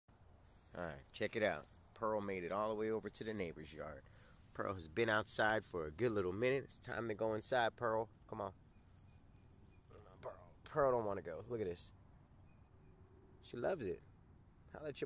purl the orange dream spider
Orange dream ball pythons are thick-bodied constrictors with triangular heads. They are harmless snakes that grow to be between four and six feet long.